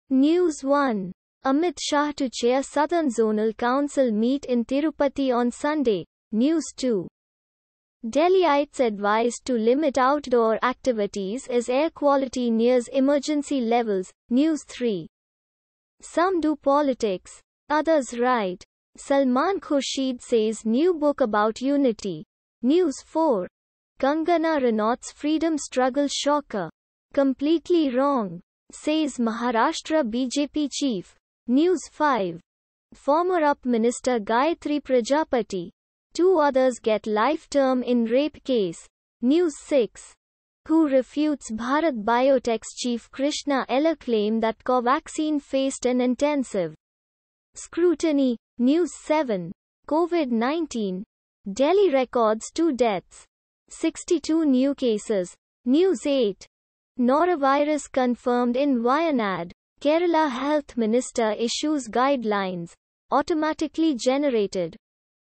News Headlines (India)- 12/11/2021,07:15 PM UTC
News 1:Amit Shah to chair Southern Zonal Council meet in Tirupati on Sunday
News 2:Delhiites advised to limit outdoor activities as air quality nears emergency levels
News 3:'Some do politics, others write': Salman Khurshid says new book about unity
News 4:Kangana Ranaut’s freedom struggle shocker: Completely wrong, says Maharashtra BJP chief
News 5:Former UP Minister Gayatri Prajapati, 2 Others Get Life Term In Rape Case
News 6:WHO refutes Bharat Biotech’s chief Krishna Ella claim that Covaxin faced an intensive scrutiny
News 7:Covid-19: Delhi records two deaths, 62 new cases
News 8:Norovirus Confirmed In Wayanad, Kerala Health Minister Issues Guidelines

Automatically Generated...